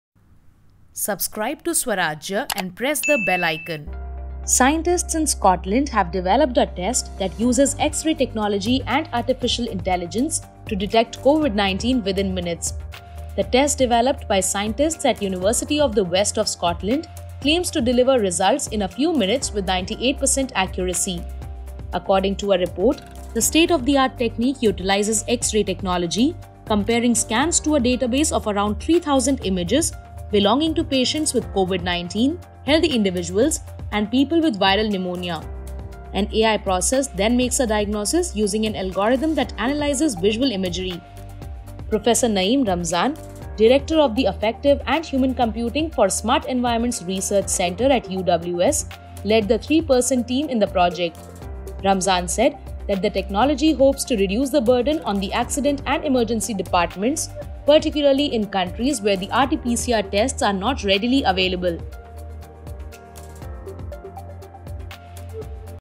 Scotland Scientists Develop Covid Test Using X-Ray And AI That Gives Result Within Minutes
Scientists in Scotland have developed a test that uses X-ray technology and Artificial Intelligence (AI) to detect coronavirus disease (Covid-19) within minutes.

While an RT-PCR test takes two hours to detect Covid-19, the test developed by scientists at University of the West of Scotland claims to deliver results in a few minutes with 98 per cent accuracy.

“The state-of-the-art technique utilizes X-ray technology, comparing scans to a database of around 3,000 images, belonging to patients with Covid-19, healthy individuals and people with viral pneumonia,” according to Medical News, an open-access medical and life sciences hub.

Also Read |How many times can one get reinfected with Covid-19?
An AI process, known as deep convolutional neural network, then makes a diagnosis using an algorithm that analyses visual imagery.

Professor Naeem Ramzan, Director of the Affective and Human Computing for SMART Environments Research Centre at UWS, led the three-person team in the project, which also involved Gabriel Okolo and Dr Stamos Katsigiannis. Ramzan said the technology hopes to reduce the burden on the accident and emergency departments, particularly in countries where RT-PCR tests are not readily available.

“Several countries are unable to carry out large numbers of Covid tests because of limited diagnosis tools, but this technique utilises easily accessible technology to quickly detect the virus,” Ramzan said.

However, Covid-19 symptoms are not visible in X-rays during the early stages of infection, he added.